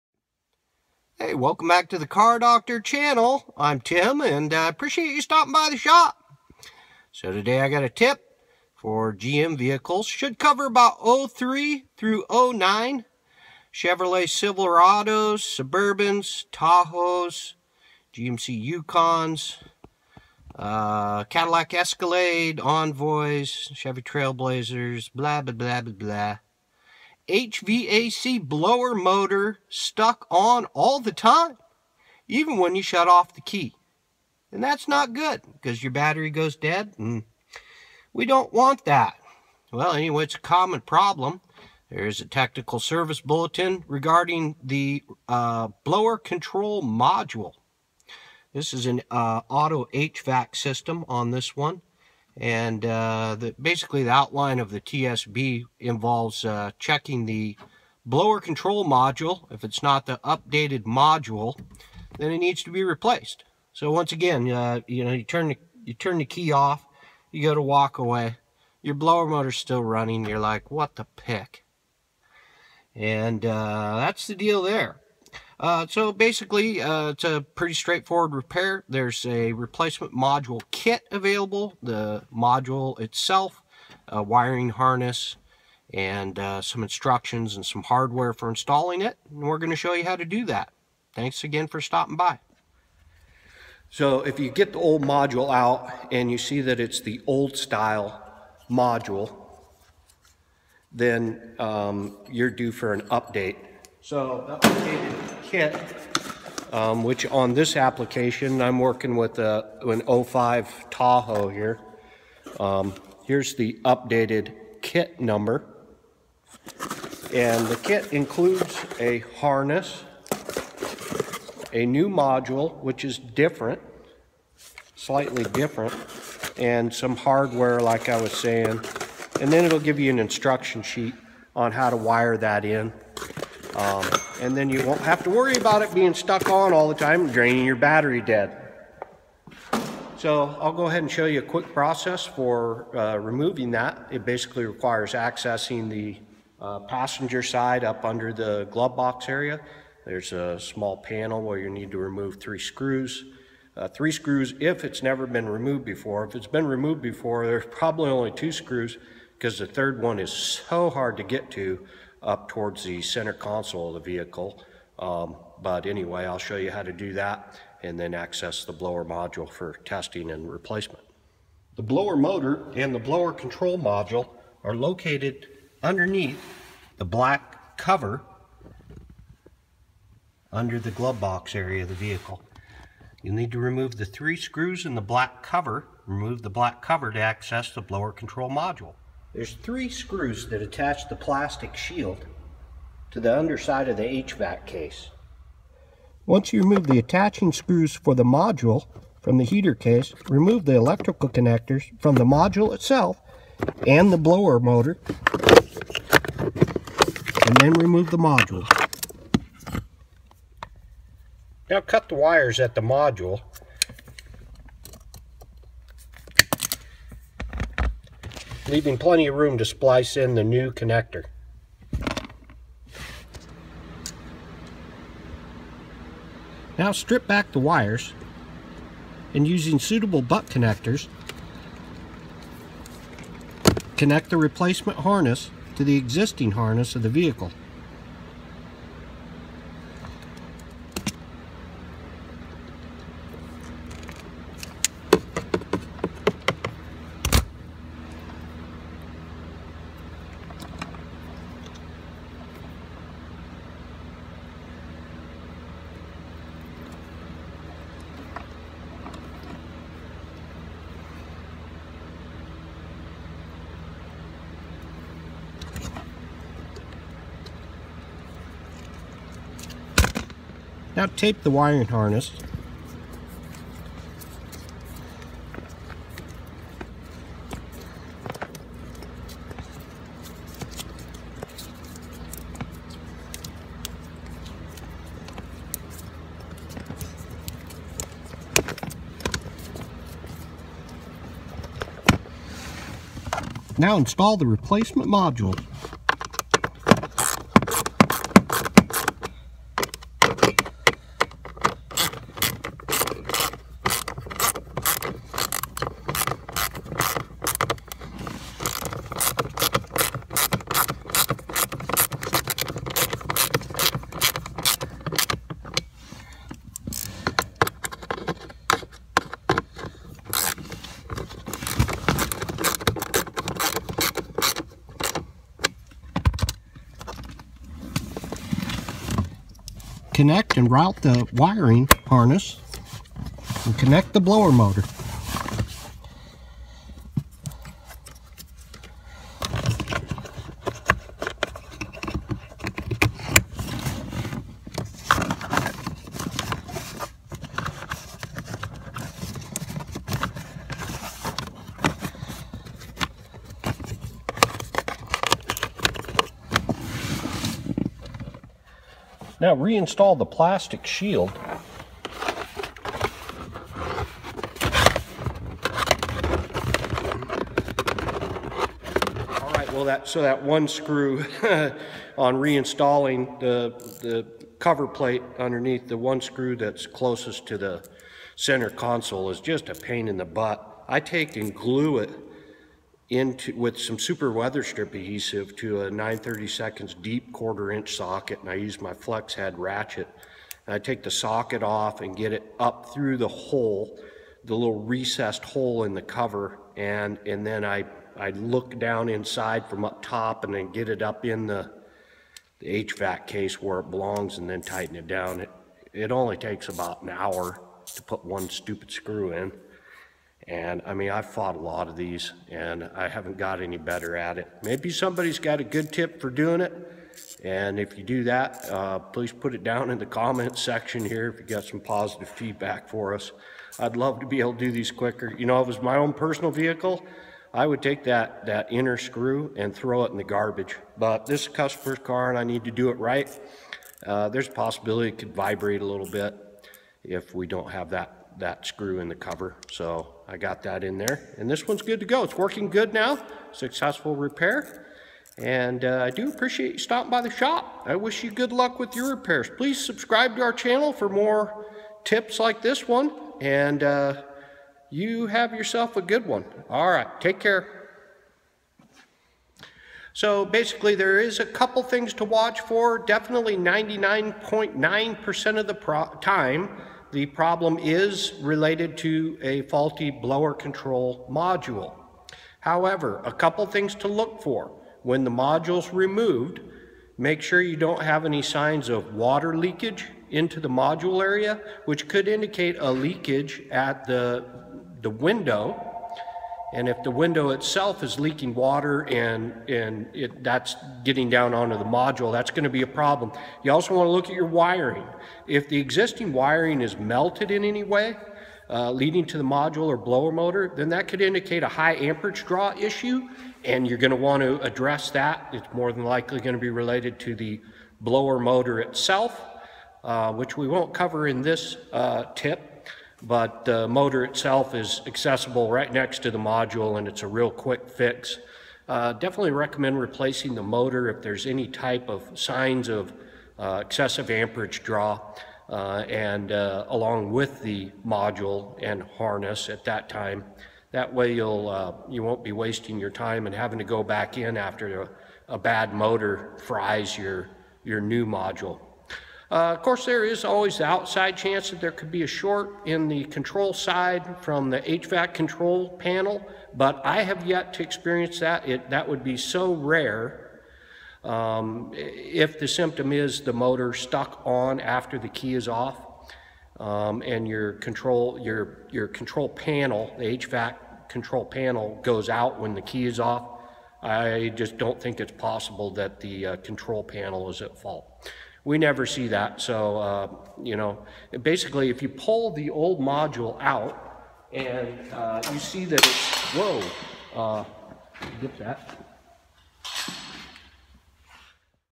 Chevy GMC Blower Fan Stuck On
Do you have a Cherolet or GMC vehicle with a blower motor that won't turn off even after removing the key? Well then this video is for you.